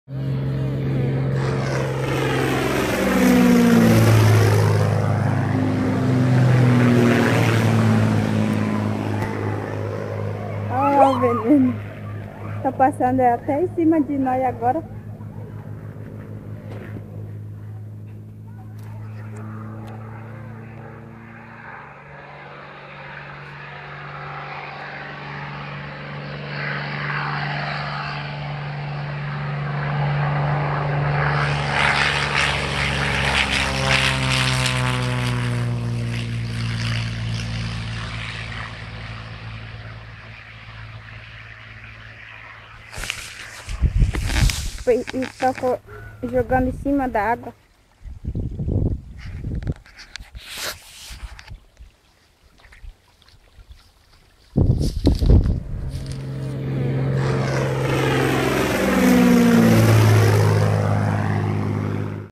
Avião pulveriza agrotóxicos sobre comunidade indígena no MS
Em imagens gravadas pelos indígenas, avião joga agrotóxicos sobre comunidade no Mato Grosso do Sul. Leia reportagem completa

Imagens: Cedidas pela comunidade Laranjeira Nhanderu
Roteiro: Ana Aranha e João Cesar Diaz
Edição: João Cesar Diaz
Mixagem de som: João Vítor Muçouçah